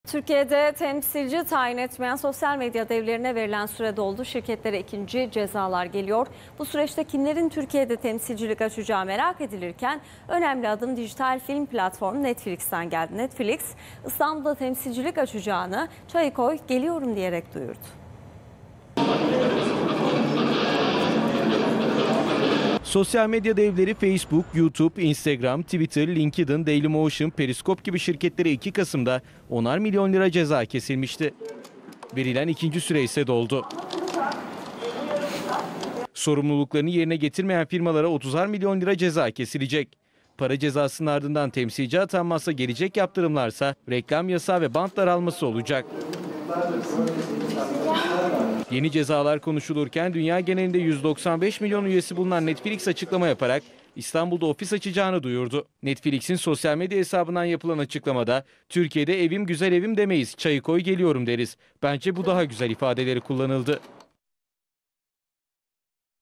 Sosyal medya şirketlerine 30'ar milyon lira ceza yolda
Şirketlerin her birine temsilci atamamasıyla 10 milyon lira ceza kesilmişti. Şimdi ise yaptırımlarda ikinci aşamaya geçiliyor. 30 milyon lira ceza yolda